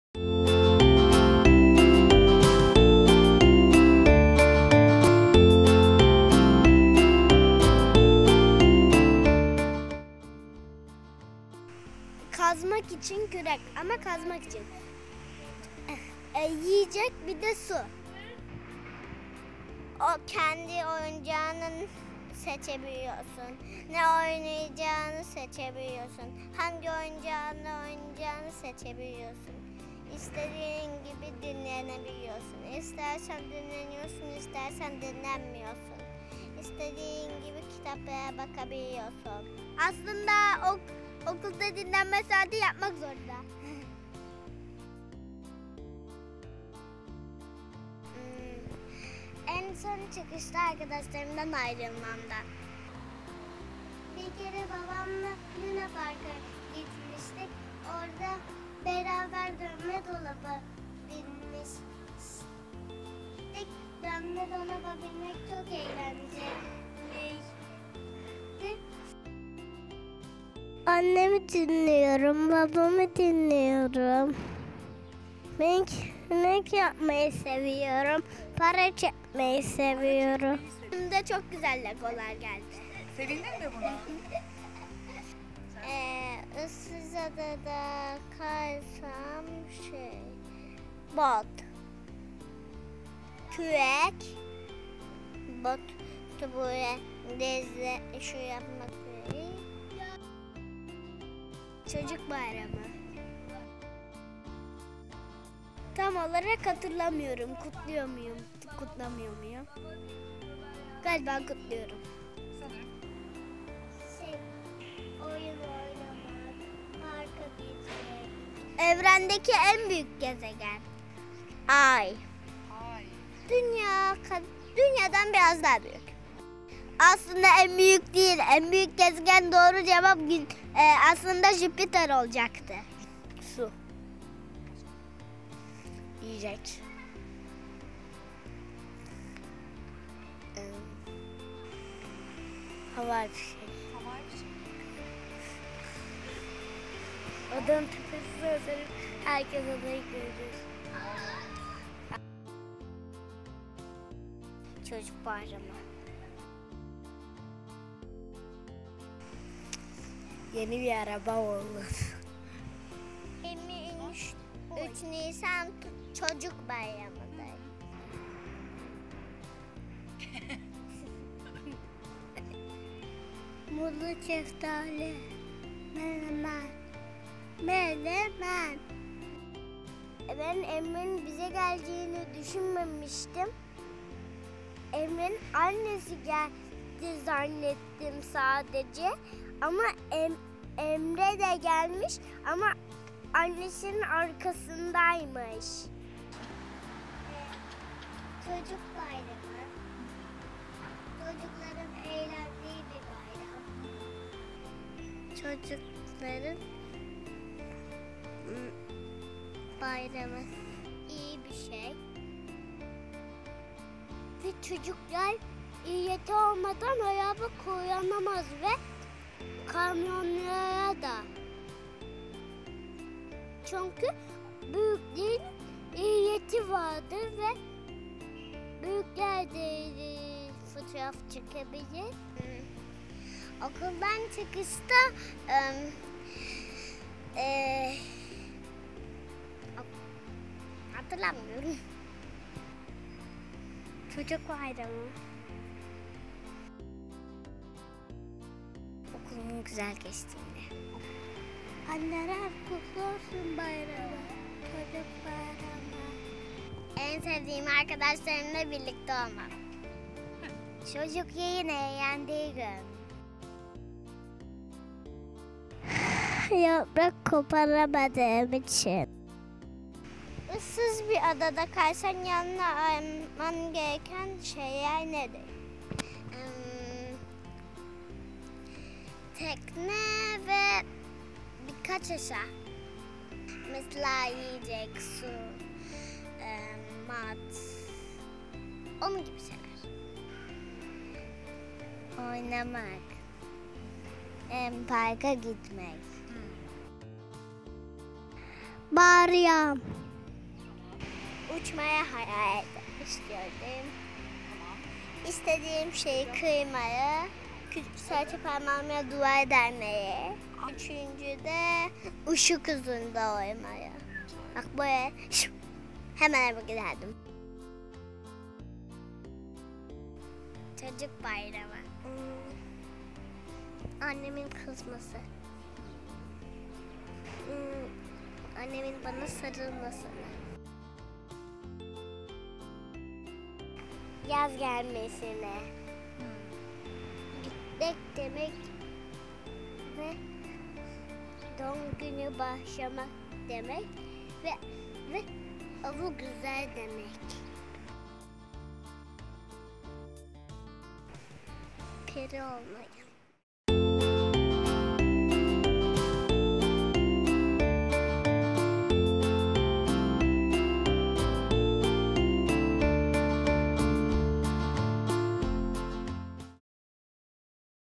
23 Nisan'ı çocuklara sorduk
23 Nisan'ı Küçük Kara Balık Anaokulu'ndaki miniklere sorduk. İlginç cevaplar duymaya hazır olun!